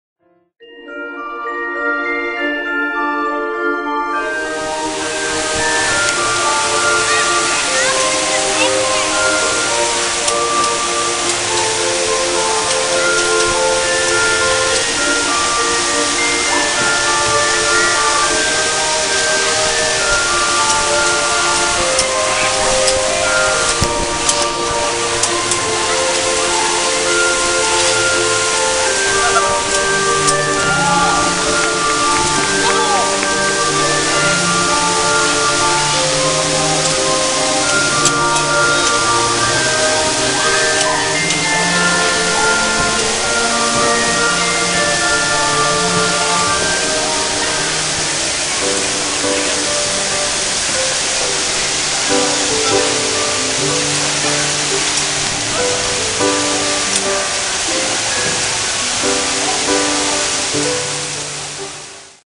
The Rink @ Rockefeller Center
October 19, 2013 - A night skate at The Rink (Iceskating , New York City's Rockefeller Center)